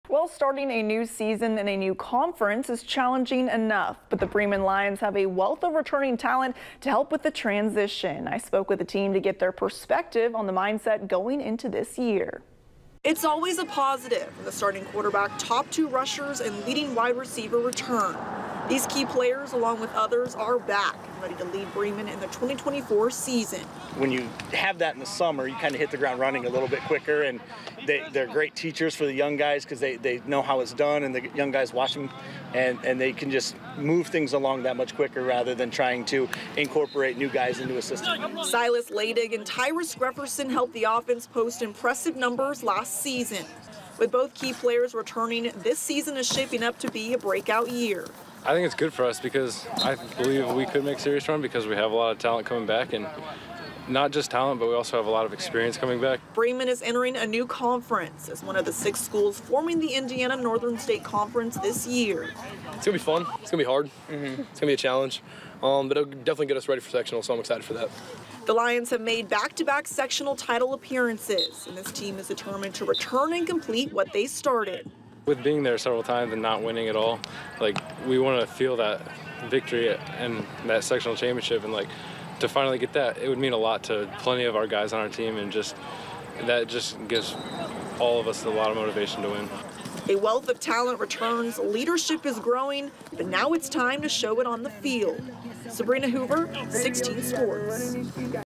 Bremen brings wealth of talent back in 2024
Starting a new season in a new conference is challenging enough, but the Bremen Lions have a wealth of returning talent to help with the transition.